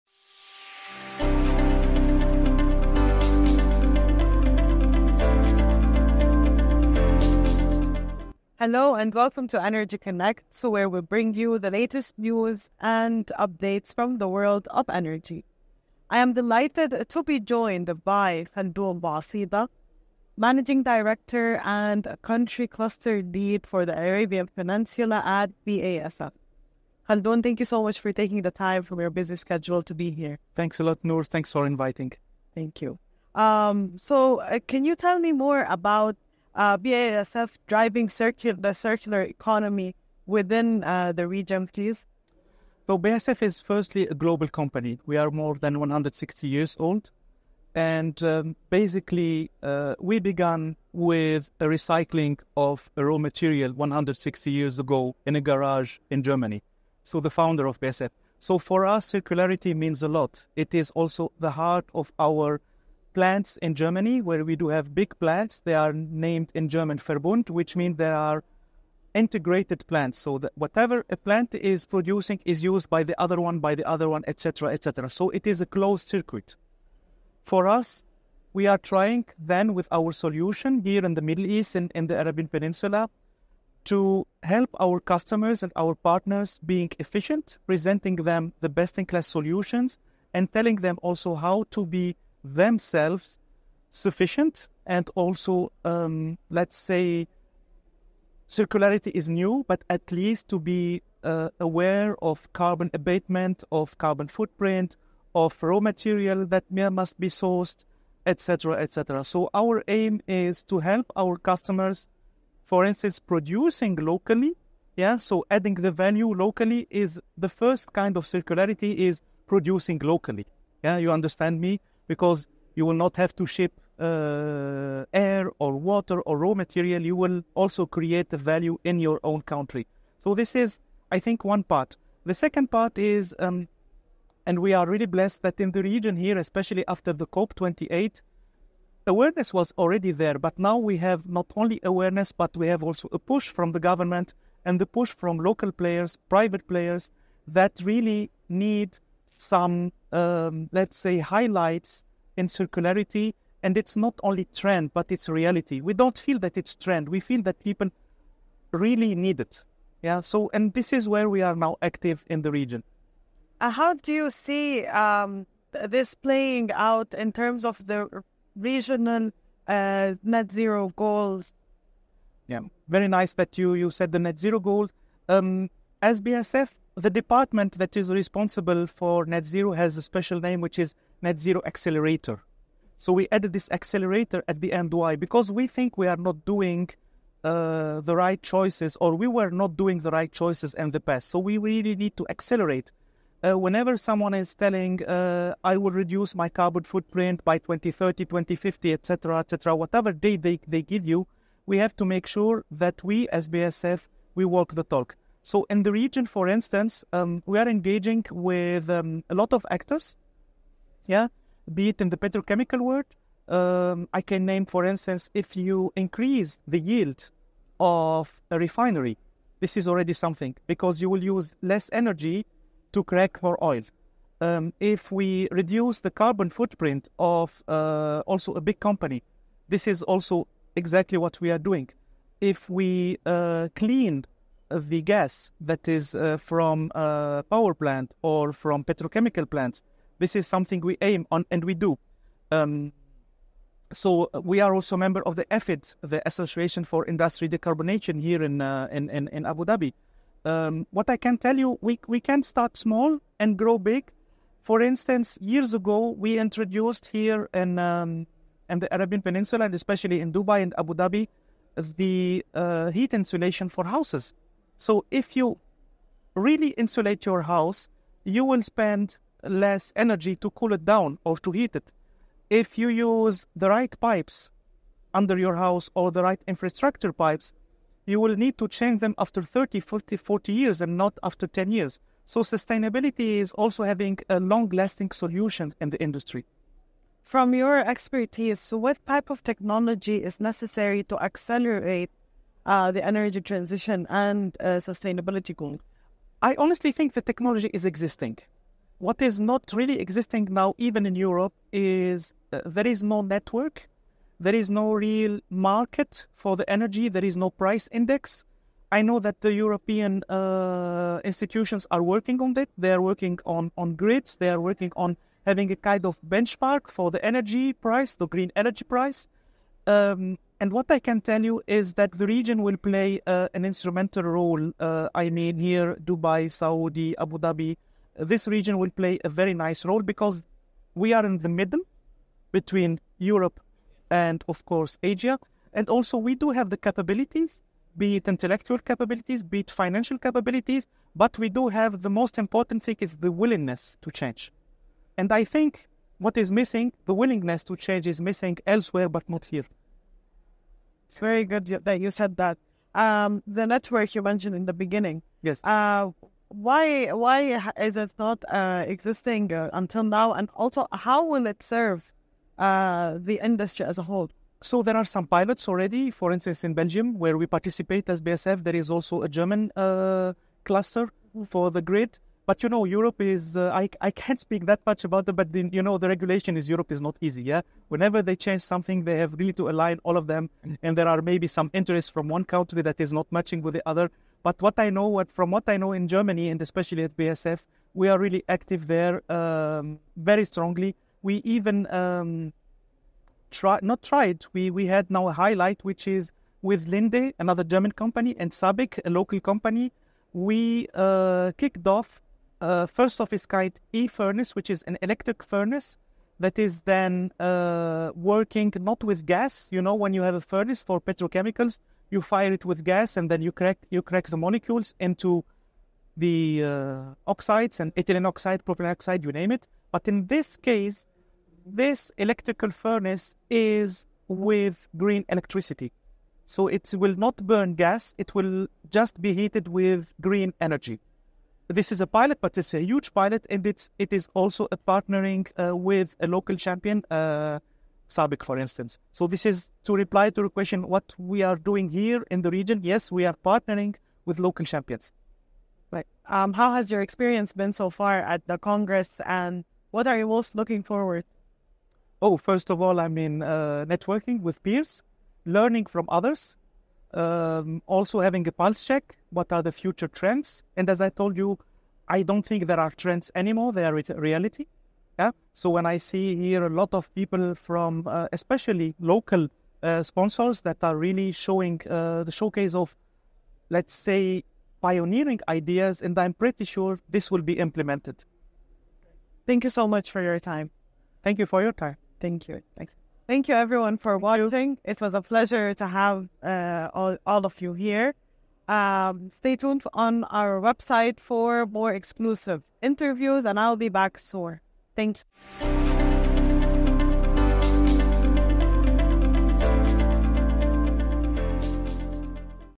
Advancing the circular economy to meet net-zero goals
In an exclusive studio interview with Energy Connects, Khaldoun Bouacida, Managing Director and Country Cluster Head for BASF, discussed BASF’s global commitment to advancing the circular economy and addressing the urgent need for sustainable practices. He delved into the company’s innovative solutions that reduce carbon footprints, enhance resource efficiency, and promote local production, all while supporting net-zero goals. Bouacida emphasised the importance of strategic partnerships—both with regional players and global leaders—to foster impactful, long-term change.